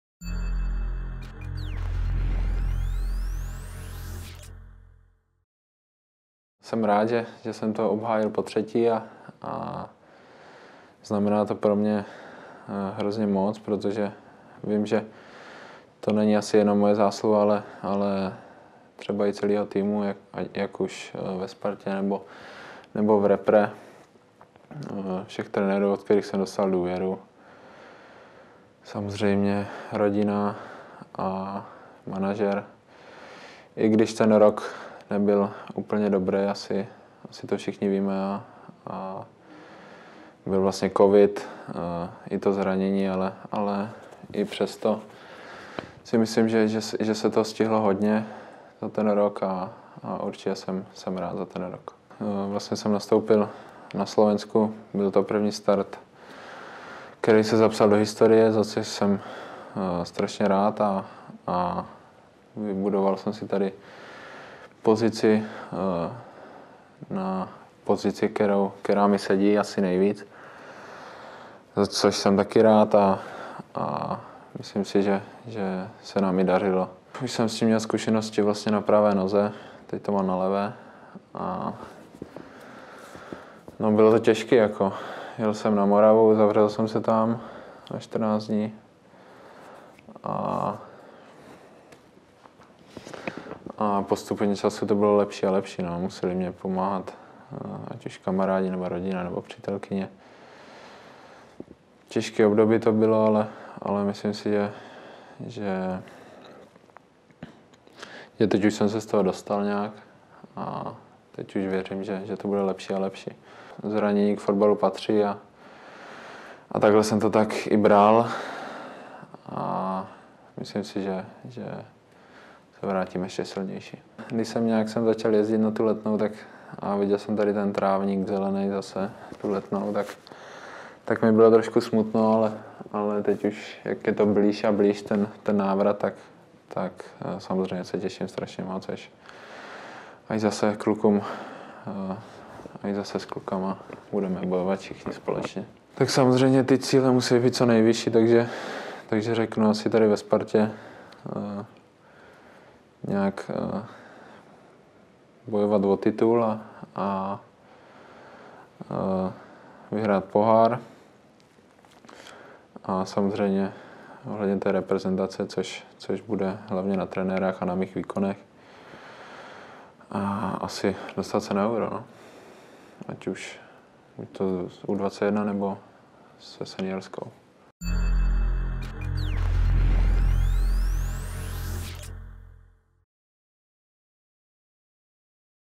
Hložek: Obhajoby prvenství si vážím, už se těším zpět na hřiště
Tři trofeje za poslední tři roky. Talentovaný reprezentant Adam Hložek obhájil prvenství v kategorii nejlepší dorostenec za rok 2020 a navázal v rámci Udělování cen XIX. Galavečera našeho fotbalu na předchozí úspěchy. Rozhovor i kompletní info o vyhlašování cen najdete na fotbal.cz.